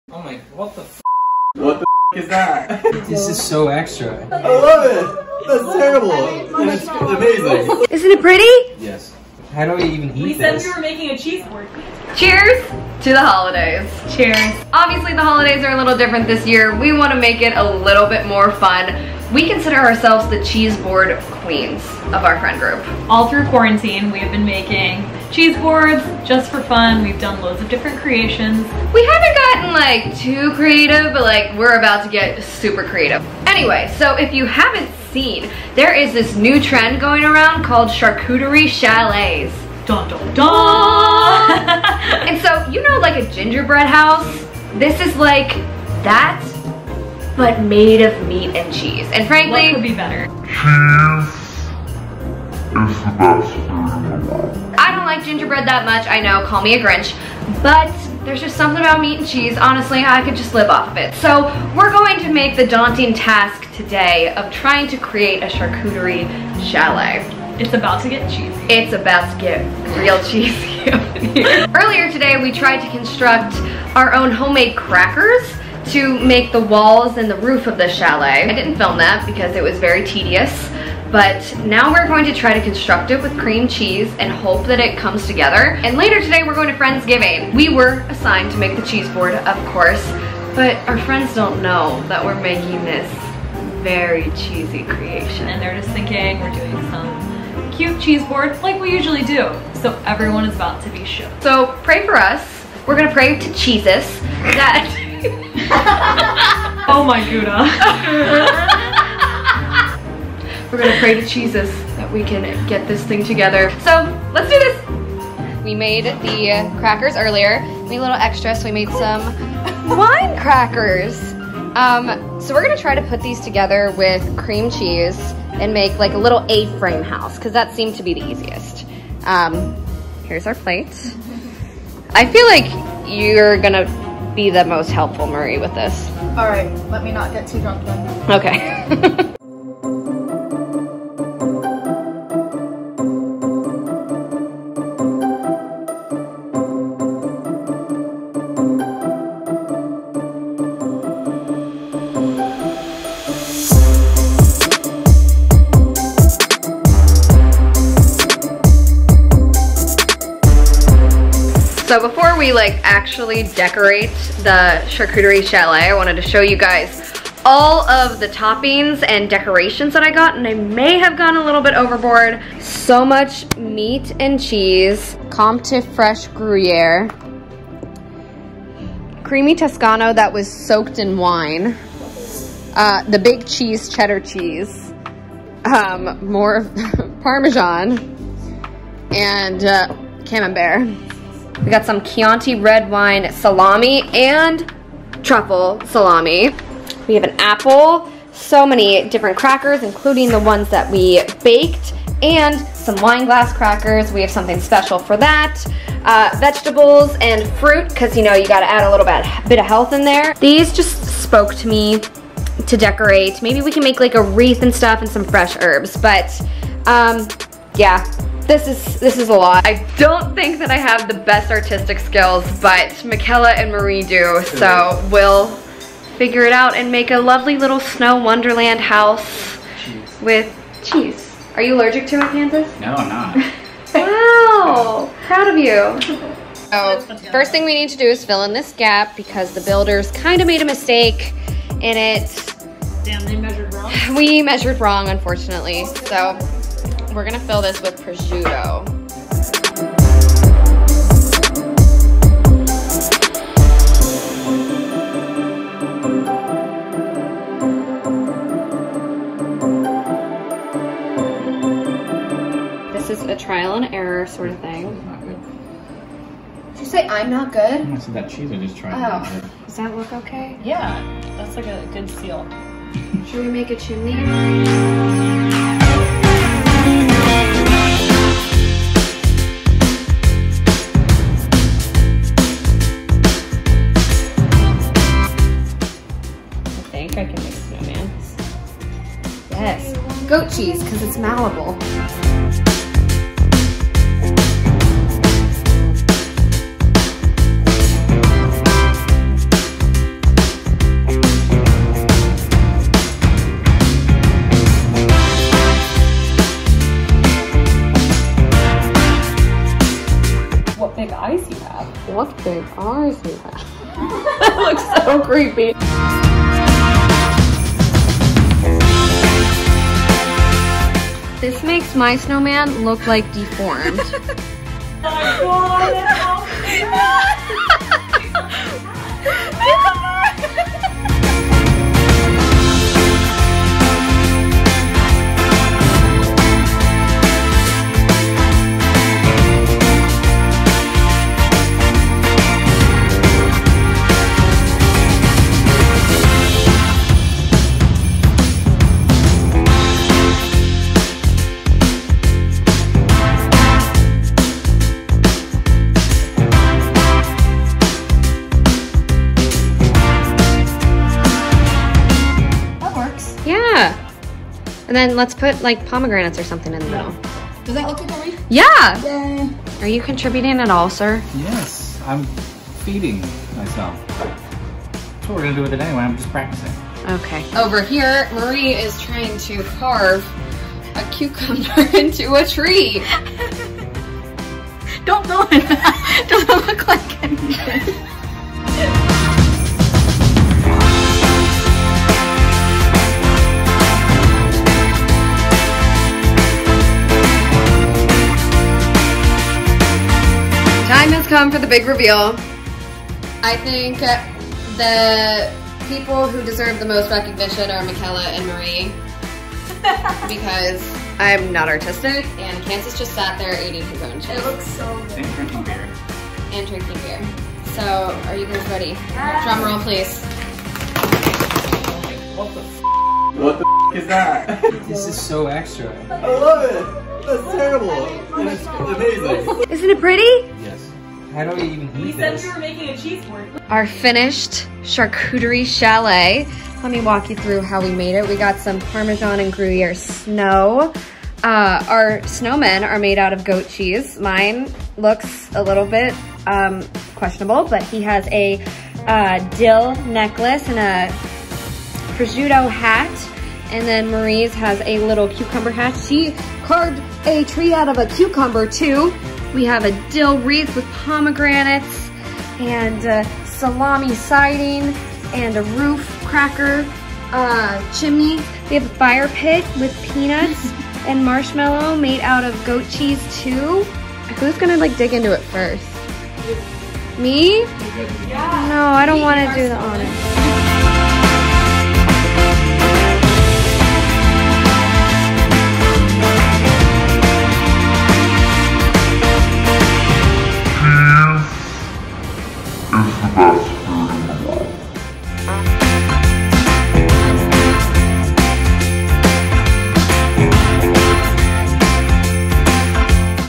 *We Made a Charcuterie Chalet!!!*
Ah yes, the time of year to build gingerbread houses is upon us, right? WRONG! This year we're making charcuterie chalets ONLY.

What is a Charcuterie Chalet? simply a house made of meat and cheese - aka the best thing in the world. For Friendsgiving this year, my roommate and I were assigned to make a cheeseboard (cause we're the queens of cheese, duh) but we decided to take it a step further by constructing this masterpiece. How do you think it turned out?